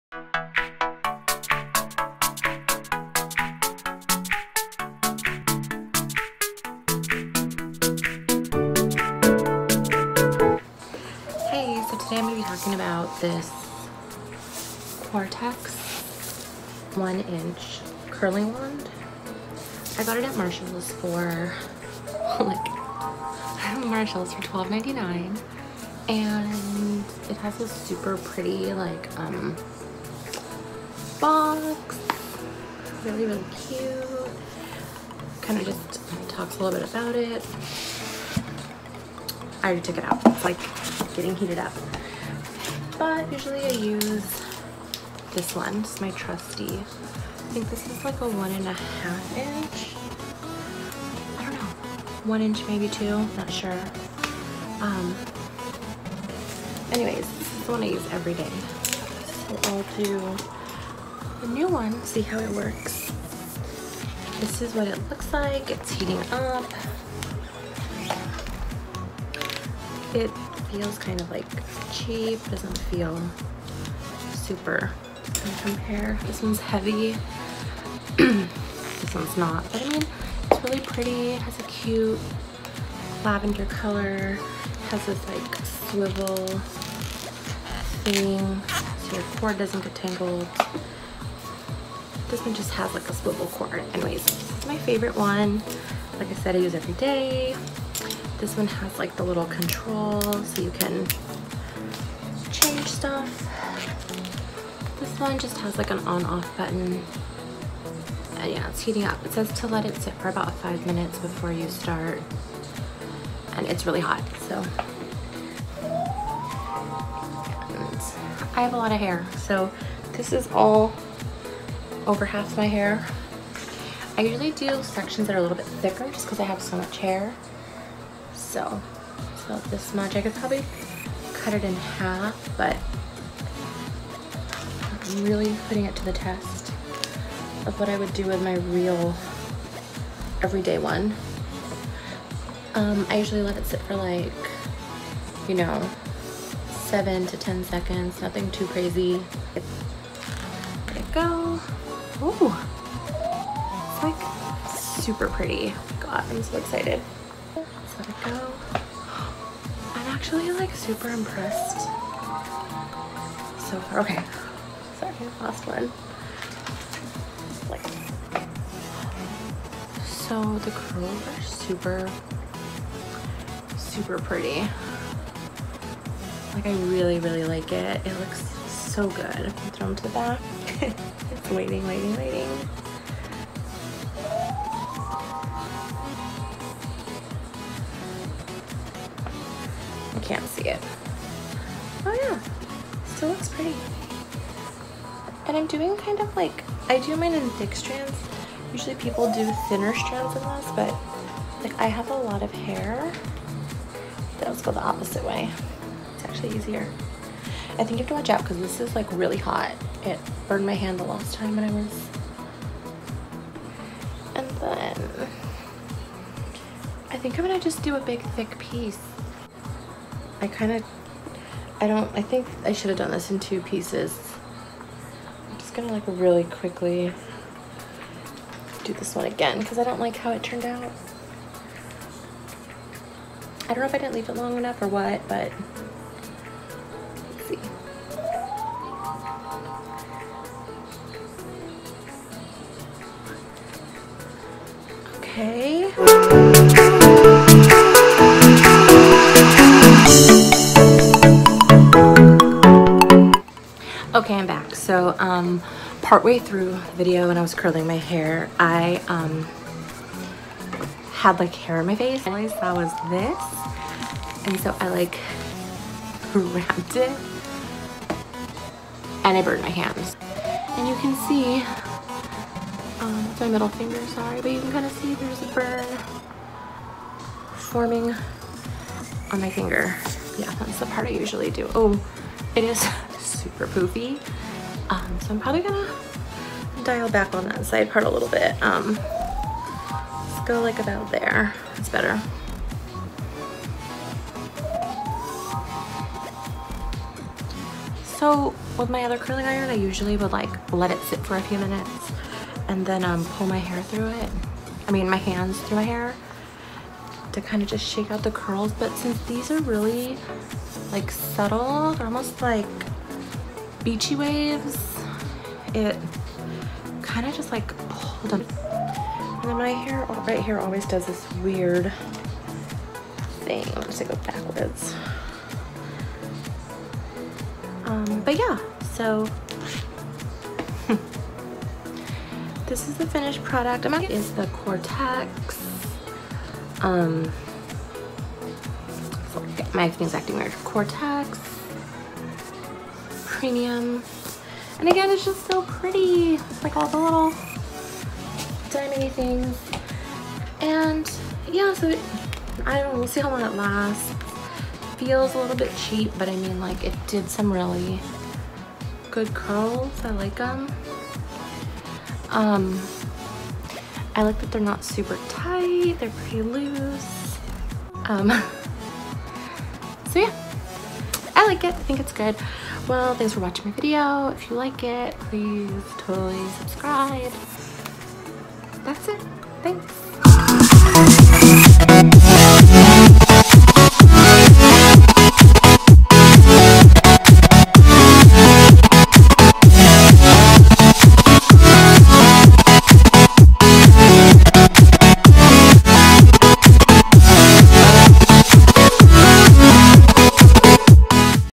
Waves for Days- 1” Curling Wand ~ Brianne Bayuga
Hi everyone & Welcome to my video!

I found this 1” Cortez graduated curling wand at Marshalls for only $12.99 so I had to buy & try.
THe goal was to have this replace my current 1 1/2” Conair curling iron.
I dont think its a replacement but definitely something I can use to shake up my hair routine!!!
So technically this is my first. Vid. So forgive my awkward cuts & that makeup under my reddish eyes... I haven’t been sleeping lol!

Make sure to join me on Instagram for fun inspo & my silly stories @Bri_Bayuga
But first let’s become besties on YouTube first, after all that’s how we met